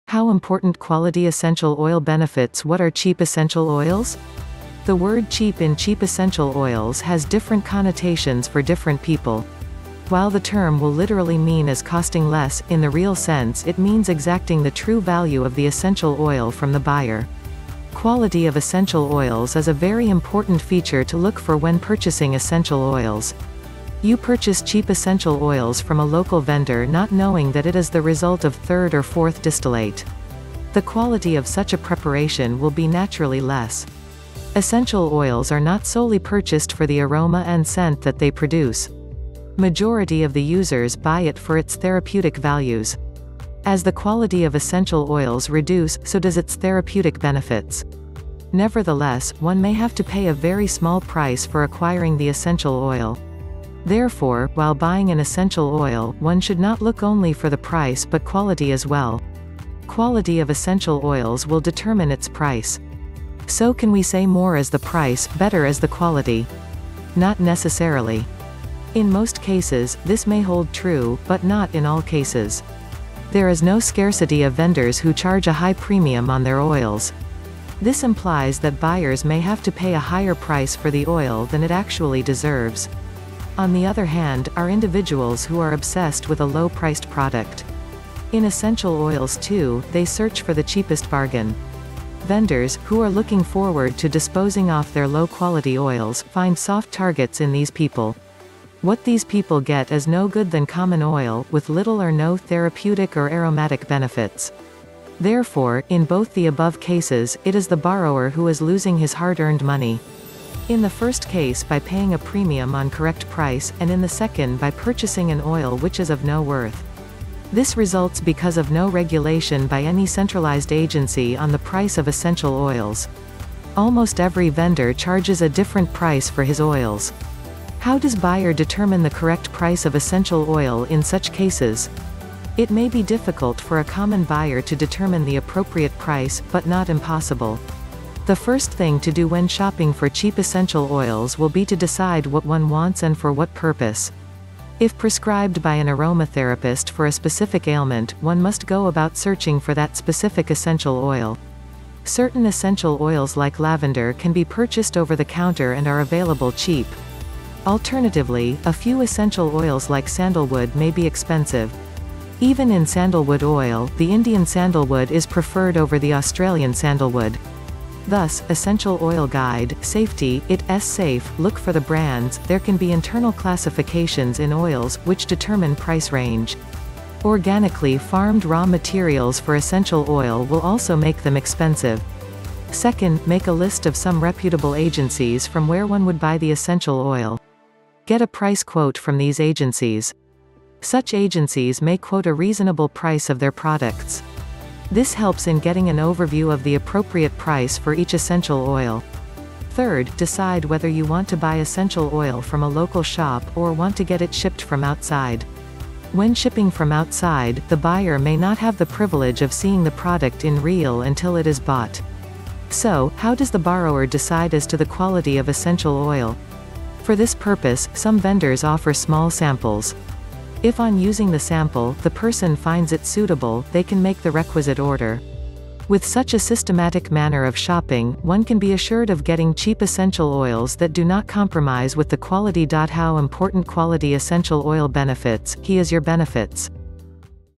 how important quality essential oil benefits
What are cheap essential oils? The word cheap in cheap essential oils has different connotations for different people. While the term will literally mean as costing less, in the real sense it means exacting the true value of the essential oil from the buyer.

Quality of essential oils is a very important feature to look for when purchasing essential oils. You purchase cheap essential oils from a local vendor not knowing that it is the result of third or fourth distillate. The quality of such a preparation will be naturally less. Essential oils are not solely purchased for the aroma and scent that they produce. Majority of the users buy it for its therapeutic values. As the quality of essential oils reduce, so does its therapeutic benefits. Nevertheless, one may have to pay a very small price for acquiring the essential oil.

Therefore, while buying an essential oil, one should not look only for the price but quality as well. Quality of essential oils will determine its price. So can we say more is the price, better is the quality. Not necessarily. In most cases, this may hold true; but not in all cases. There is no scarcity of vendors who charge a high premium on their oils. This implies that buyers may have to pay a higher price for the oil than it actually deserves.

On the other hand, are individuals who are obsessed with a low priced product. In essential oils too, they search for the cheapest bargain. Vendors, who are looking forward to disposing off their low quality oils, find soft targets in these people. What these people get is no good than common oil,  with little or no therapeutic or aromatic benefits.

Therefore, in both the above cases, it is the borrower who is losing his hard-earned money. In the first case by paying a premium on correct price; and in the second by purchasing an oil which is of no worth. This results because of no regulation by any centralised agency on the price of essential oils. Almost every vendor charges a different price for his oils. How does buyer determine the correct price of essential oil in such cases? It may be difficult for a common buyer to determine the appropriate price, but not impossible.

The first thing to do when shopping for cheap essential oils will be to decide what one wants and for what purpose. If prescribed by an aromatherapist for a specific ailment, one must go about searching for that specific essential oil. Certain essential oils like lavender can be purchased over the counter and are available cheap. Alternatively, a few essential oils like sandalwood may be expensive. Even in sandalwood oil, the Indian sandalwood is preferred over the Australian sandalwood. Thus, essential oil guide, safety,it,s safe,look for the brands, there can be internal classifications in oils, which determine price range. Organically farmed raw materials for essential oil will also make them expensive.

Second, make a list of some reputable agencies from where one would buy the essential oil. Get a price quote from these agencies. Such agencies may quote a reasonable price of their products. This helps in getting an overview of the appropriate price for each essential oil.

Third, decide whether you want to buy essential oil from a local shop or want to get it shipped from outside. When shipping from outside, the buyer may not have the privilege of seeing the product in real until it is bought. So, how does the borrower decide as to the quality of essential oil? For this purpose, some vendors offer small samples. If on using the sample, the person finds it suitable, they can make the requisite order.

With such a systematic manner of shopping, one can be assured of getting cheap essential oils that do not compromise with the quality.how  important quality  essential oil benefits.HE is your benefits.